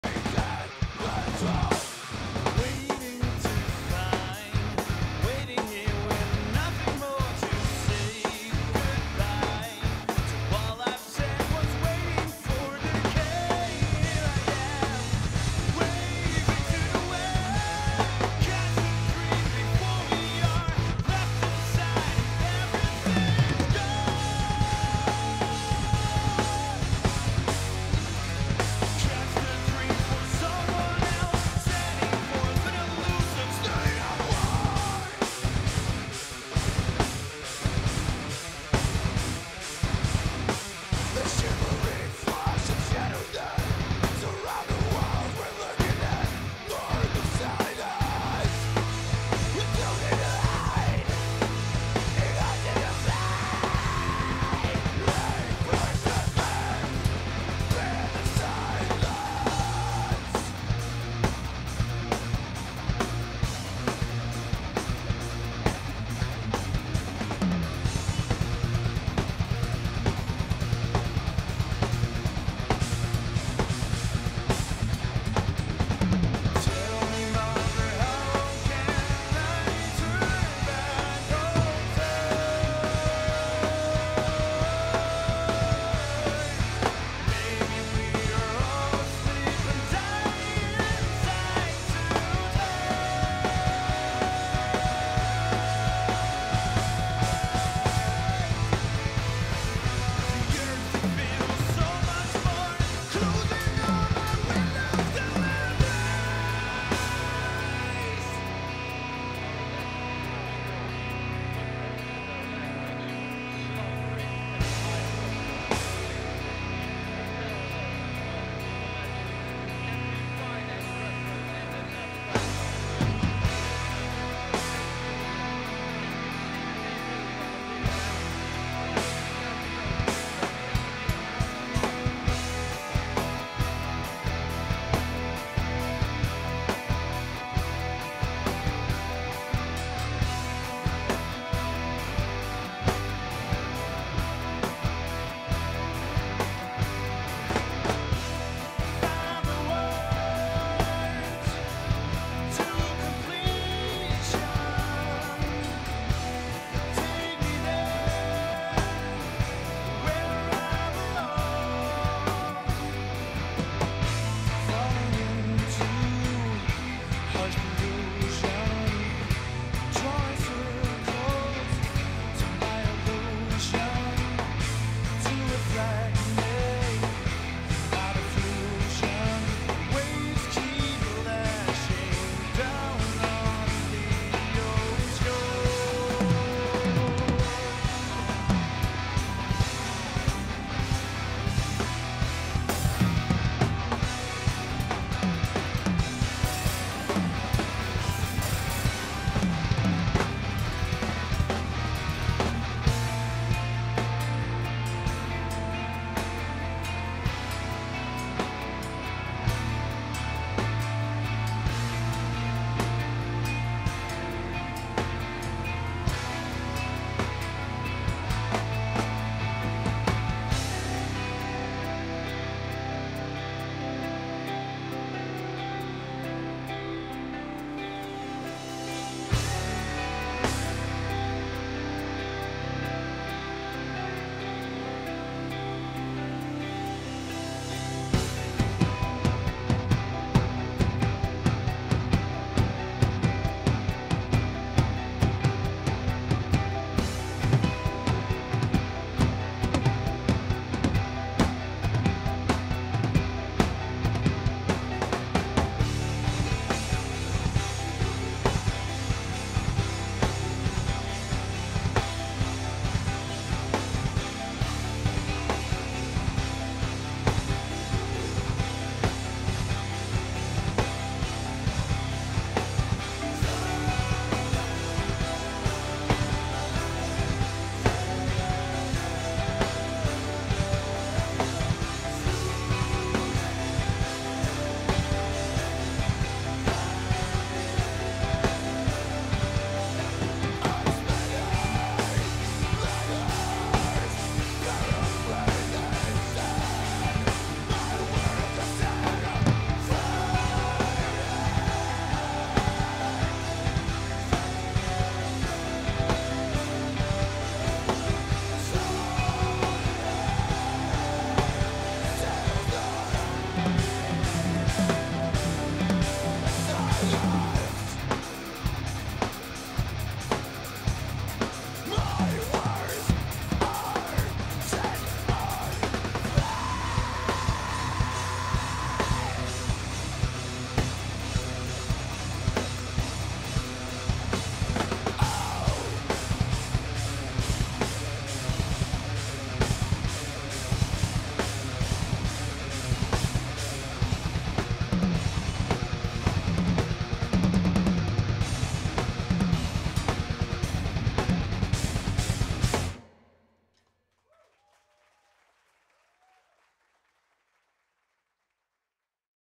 Stef Broks @ D-DAY - Drummers Meeting Meppel 2012
Textures drummer Stef Broks performing @ D-DAY - Drummers Meeting Meppel @ Rimshot Drum Centre on Saturday June 23th 2012.